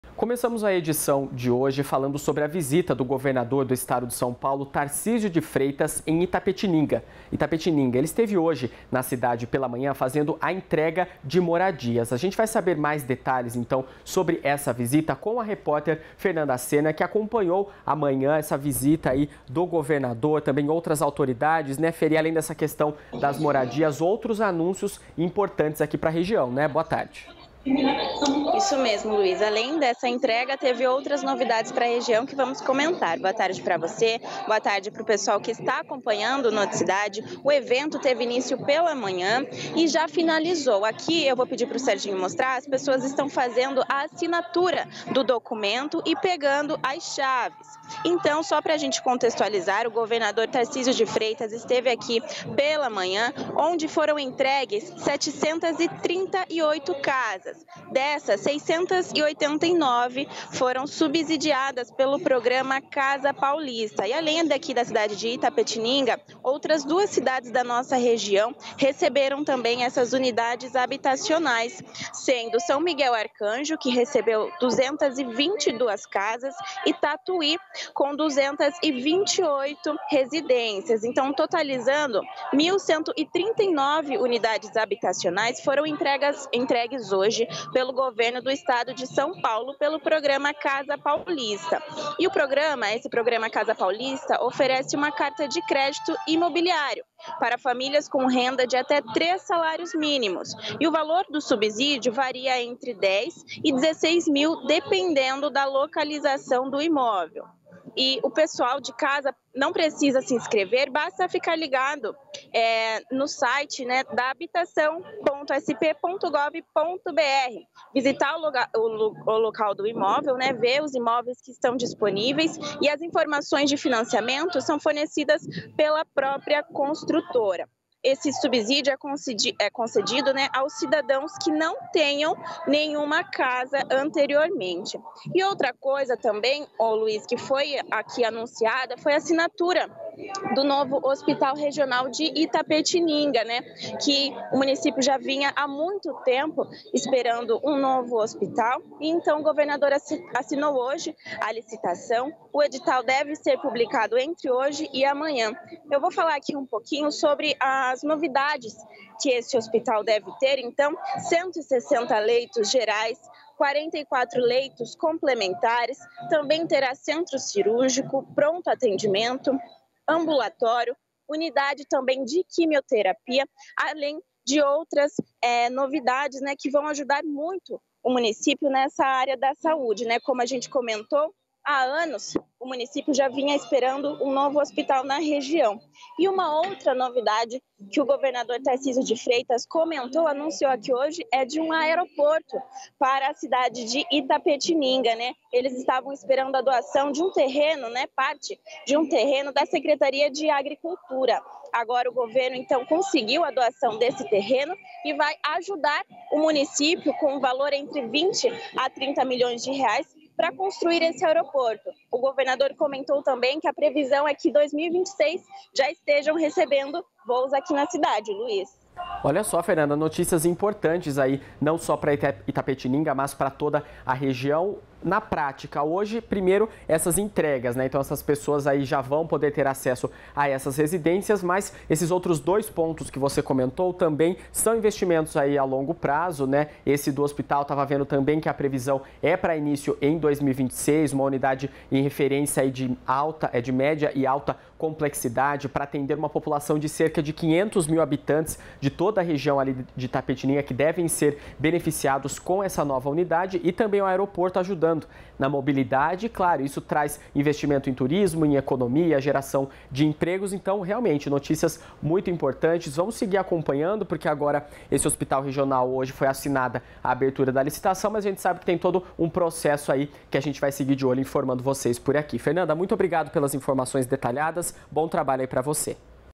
Governador entrega moradias e anuncia construção de hospital regional e aeroporto | TV Sorocaba SBT
Governador entrega moradias e anuncia construção de hospital regional e aeroporto

As novas moradias fazem parte de um esforço regional de habitação.